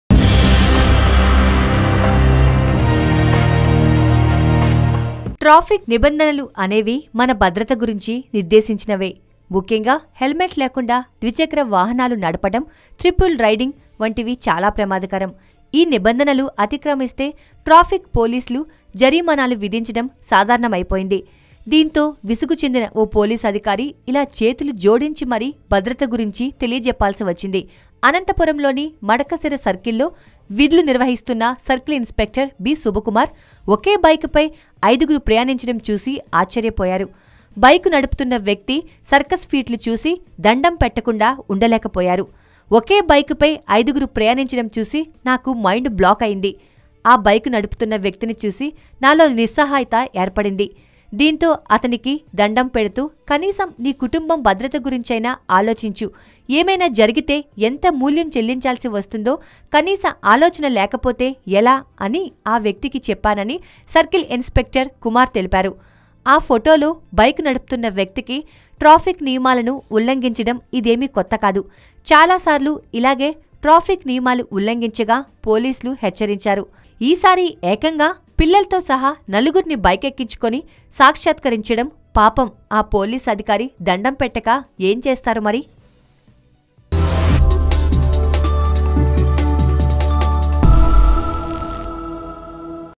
దండాలయ్యా అంటూ ఆ కుటుంబానికి నమస్కారం చేసిన పోలీస్| A P Police Officer Folding His Hand| YOYO NEWS24
A P Police Officer Folding His Hand.
దండాలయ్యా అంటూ ఆ కుటుంబానికి నమస్కారం చేసిన పోలీస్

AP Police has requesting to follow the traffic rules by folding his hands. This police image has circulating and creating sensation around the social media, A photo of police personnel folding his hands and requesting an traffic offender party on a bike with his group of five people .

Taken in Anantapur area, the photograph demonstrates a policeman arguing the man who sits on the bike alongside his three children and his better half. Every one of the five are seen without protective caps.